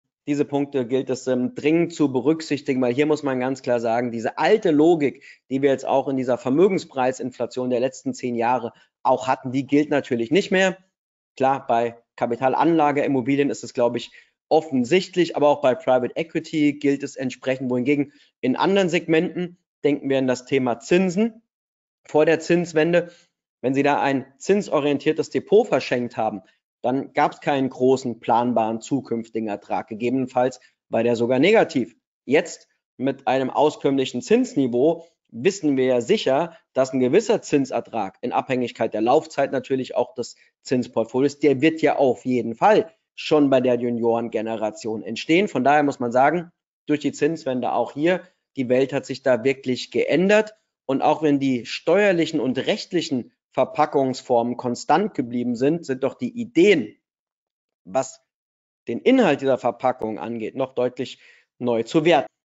Vermögensübertragung: Ganz neue Diskussionspunkte
✔Sprechen Sie uns gerne an und vereinbaren Sie ein kostenfreies & unverbindliches Erstgespräch mit einem unserer Experten!

Die Themen im Überblick:
🧠 Klassische Überlegungen mit veränderten Maßnahmen
🧠Trotz steuerrechtlich gleichbleibender Lage, haben sich die Ideen mit der Zinswende weiterentwickelt